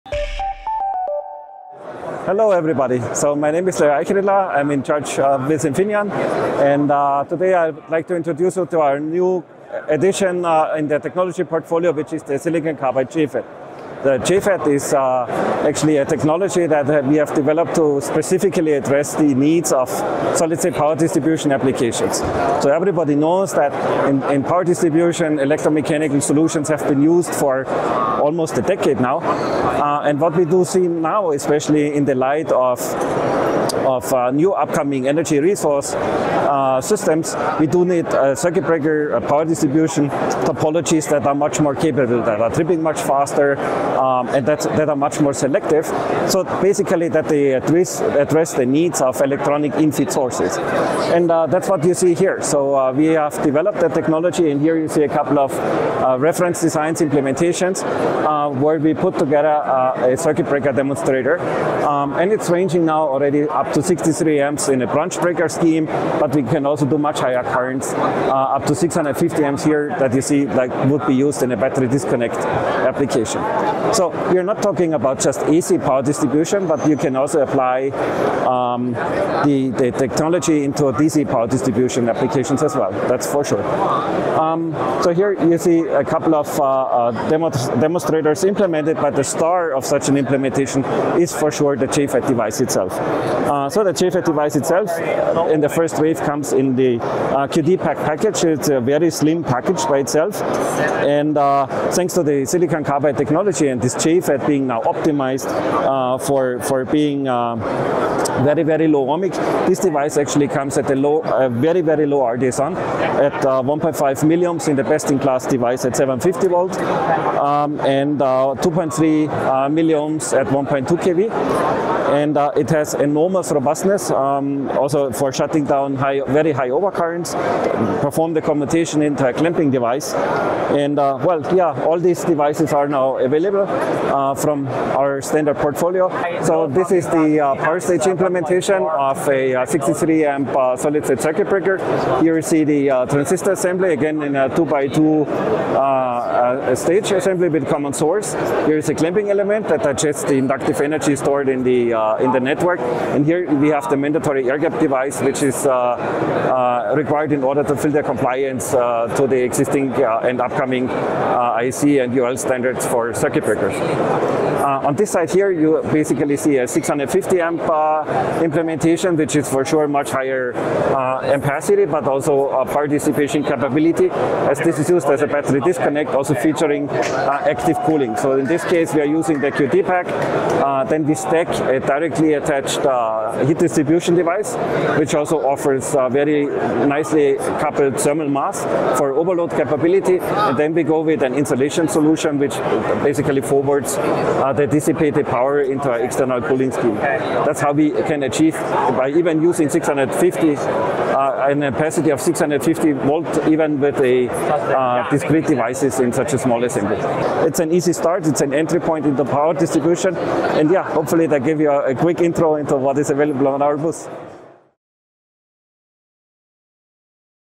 SiC JFET Empowers Next-Gen Switching Applications
Enabling solid-state circuit breakers to replace conventional electromechanical products, Infineon's CoolSiC JFET technology leverages the benefits of silicon carbide (SiC) in innovative packaging and advanced chip design.

CoolSiC JFETs challenge the industry in system efficiency, robustness, and reliable performance in the most demanding environments. In this video, Infineon’s Leo Aichriedler explains the functionality of CoolSiC JFET technology as a key component in advanced circuit-breaker solutions.